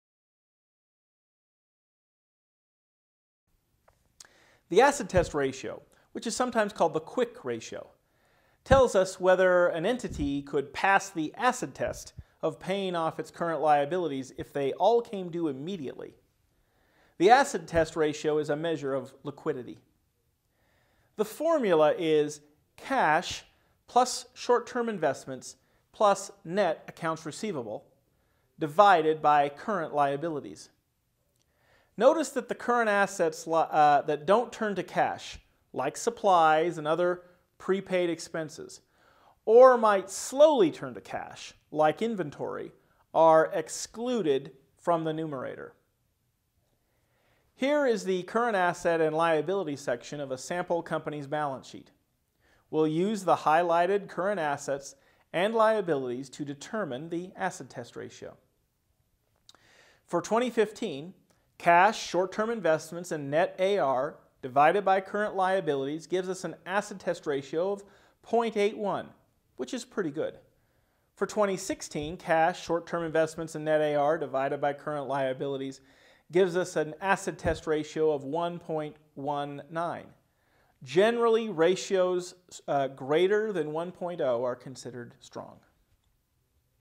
Financial Analysis: Acid-Test Ratio Example
This video introduces and includes an example of the financial statement analysis tool:
Acid-Test Ratio